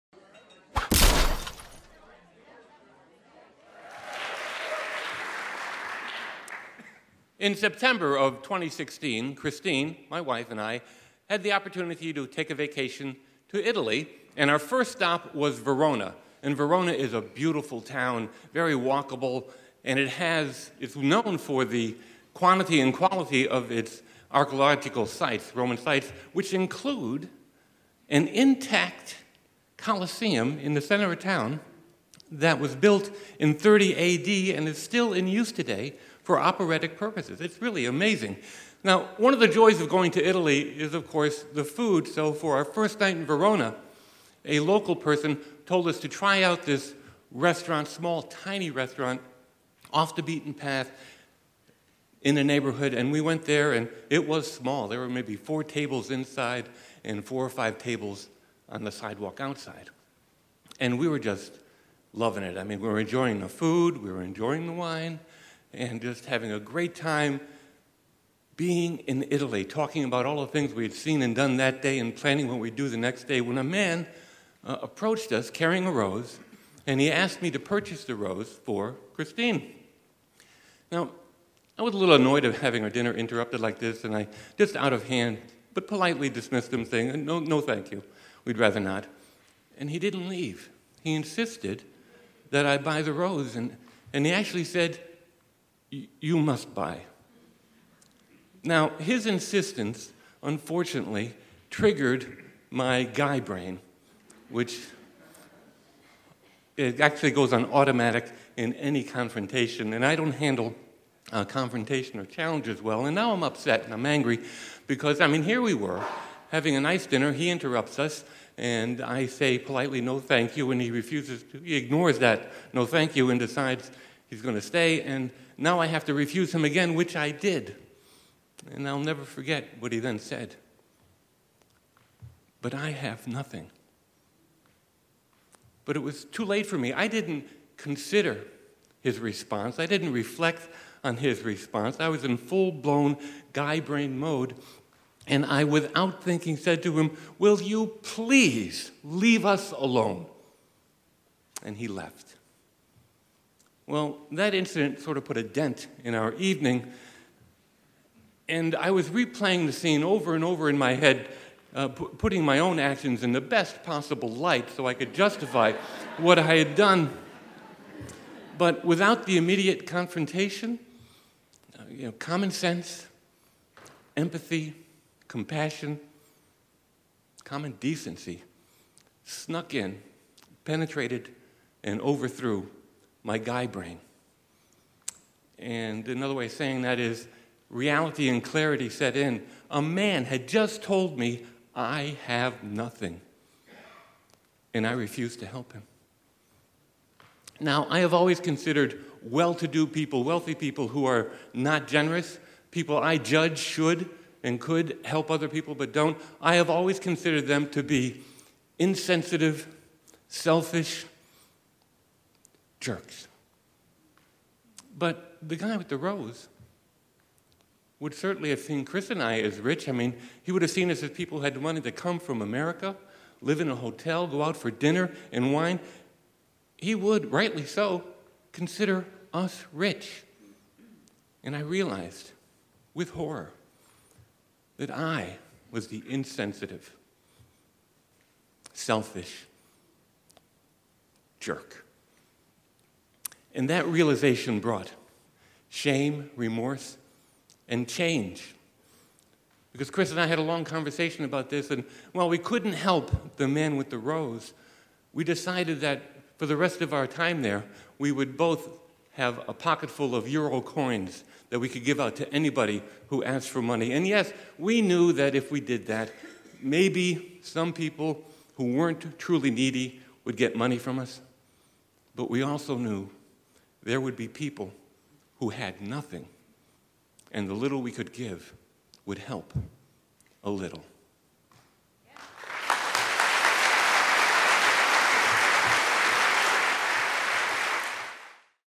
Lancaster Story Slam - Grand Slam - Changes - Tony Crocamo
Tony Crocamo tells of the trip he and his wife took to Italy in which he was shocked and ashamed of his own insensitivity and lack of compassion. The theme was “Changes.”